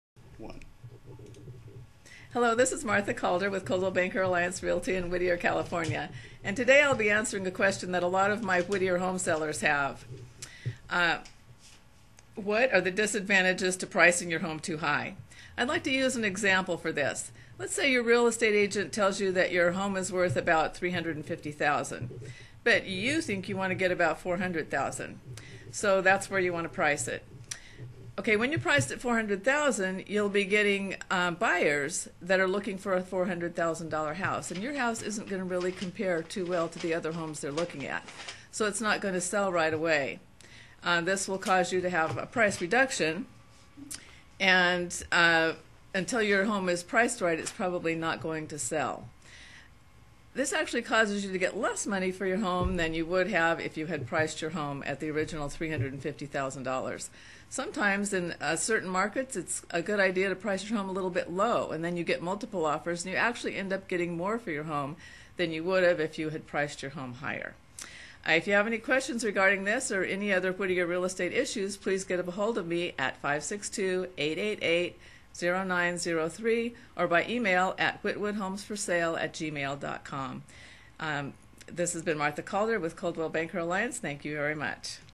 Real Estate Answer Man (Woman) - Whittier Home Seller - What's Wrong with Pricing my Home Too High?
Hello this is Martha Calder with Coldwell Banker Alliance Realty in Whittier, California and today Ill be answering a question that a lot of my Whittier Home Sellers have ...

What are the disadvantages to pricing your home too high?

I'd like to use and example for this. Lets say your Real Estate Agent tells you your home is worth about $350,000, but you think you want to get about $400,000, so that's where you want to price it. Ok, when you're priced at $400,000 you'll be getting buyers that are looking for a $400,000 dollar house and your house isn't really going to compare too well to the other houses they're looking at, so its not going to sell right away.

This will cause you to have a price reduction and until your home is priced right its probably not going to sell.

This actually causes you to get less money for your home than you would have if you had priced your home at the original $350,000 dollars. Sometimes in certain markets its a good idea to price your home a little bit low and then you get multiple offers and you actually end up getting more for your home than you would have if you had priced your home higher.

If you have any questions regarding this or any other Whittier Real Estate issues you can get a hold of me at or by email at

This had been Martha Calder at Coldwell Banker Alliance.

Thanks Martha! =)

Coach